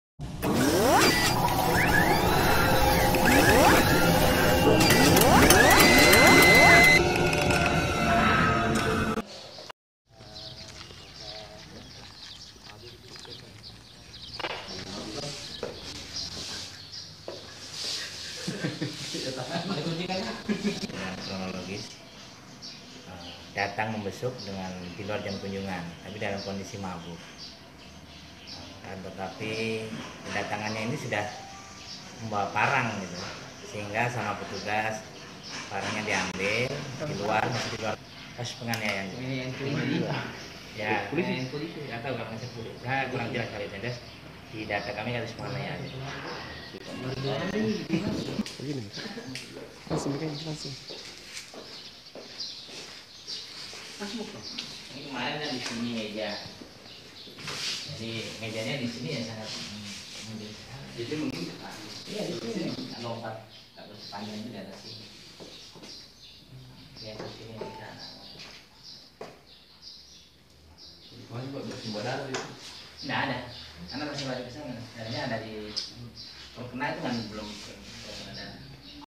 Pemarangan Petugas Rutan Masamba oleh Residivis yang Baru 2 Hari Bebas
MASAMBA, KLIKNEWS.ID ­­­ Residivis kasus penganiayaan, yang berinisial R (19), kembali melakukan penganiayaan kepada salah seorang petugas Rutan di pintu masuk Rumah Tanahan (Rutan) kelas II B Masamba, yakni Abd. Asiz, Senin 23 Juli
2016.